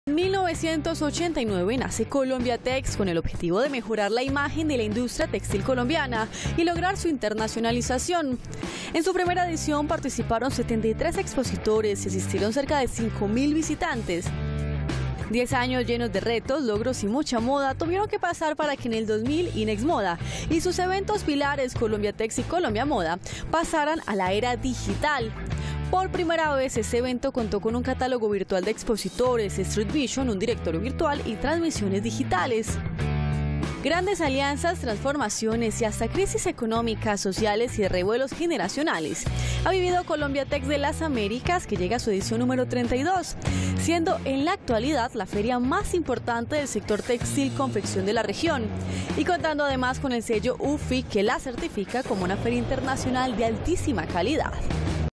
Colombiatex, una feria con 32 años de historia en el sector textil [Noticias] - Telemedellín
En 1989 nace Colombiatex con el objetivo de mejorar la imagen de la industria textil colombiana y lograr su internacionalización.